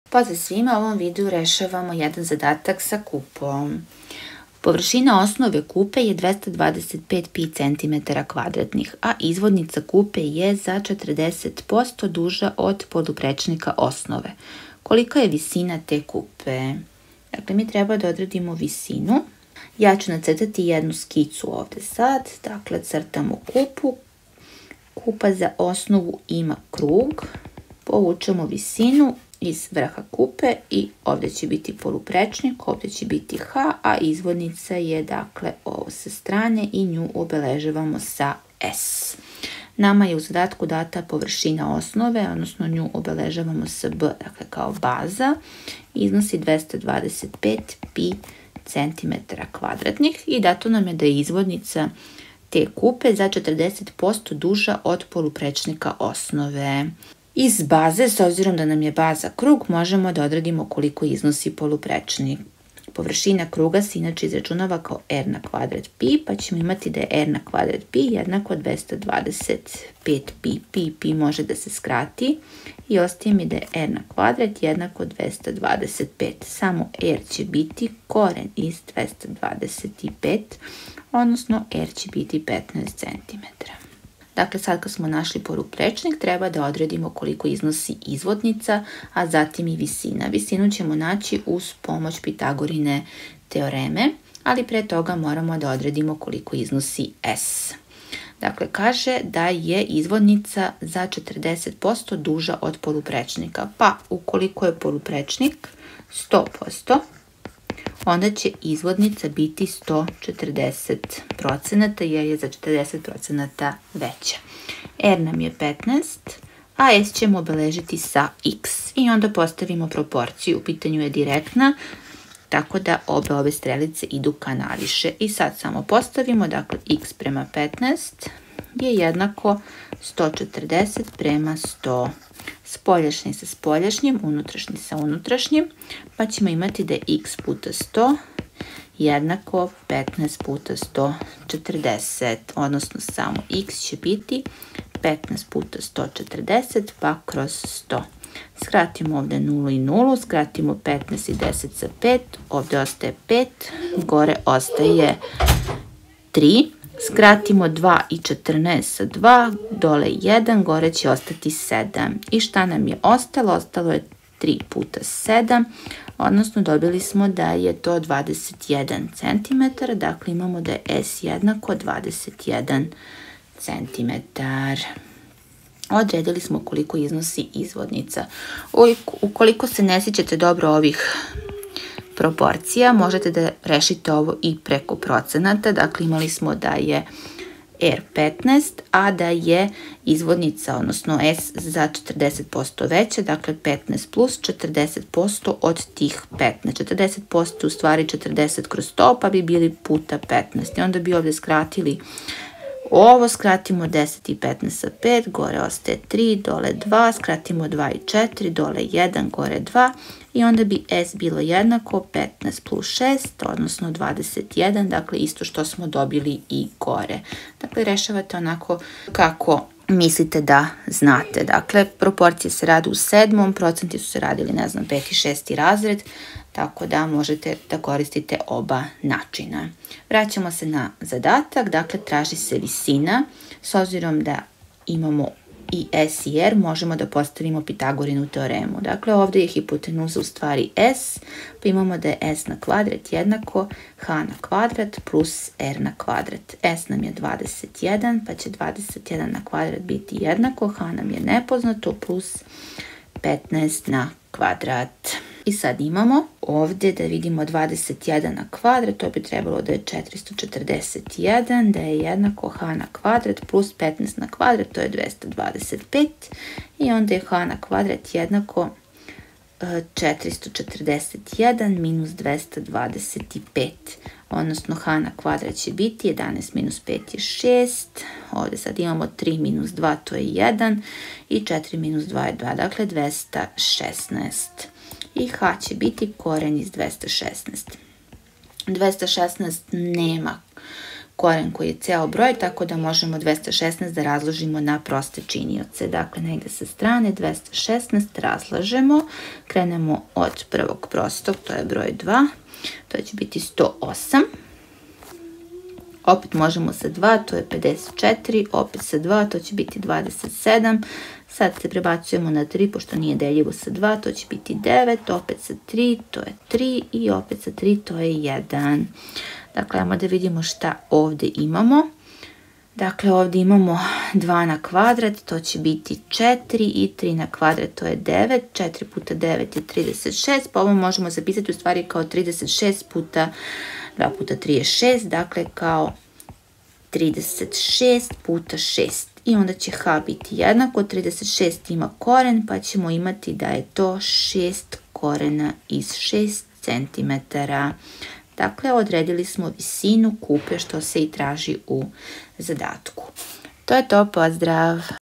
Kupa zadaci za 8 razred
Kupa zadaci za 8 razred. U ovoj lekciji rešavamo zadatak iz oblasti kupa.